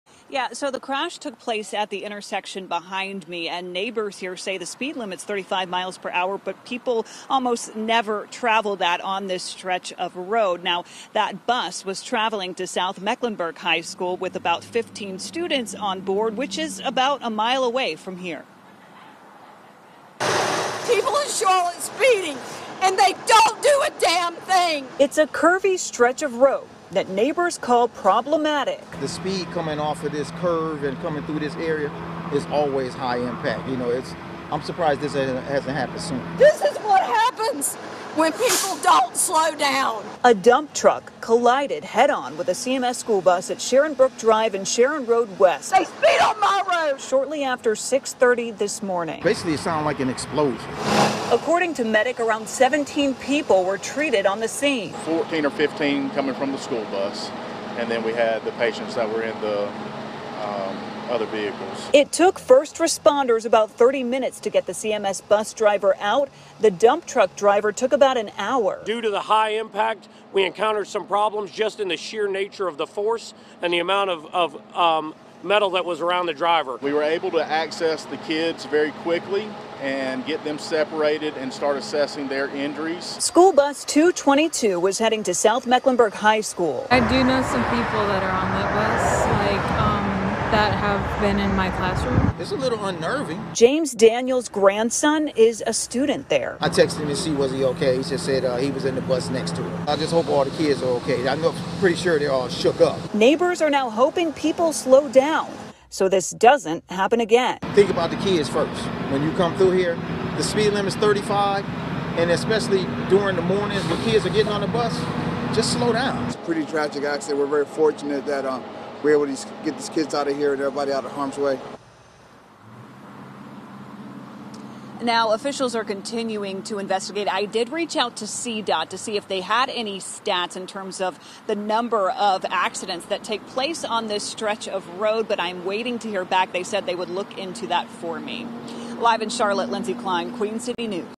Roadway near CMS bus wreck 'problematic': Neighbors
A school bus carrying students to South Mecklenburg High School was involved in a serious head-on crash in south Charlotte Wednesday morning.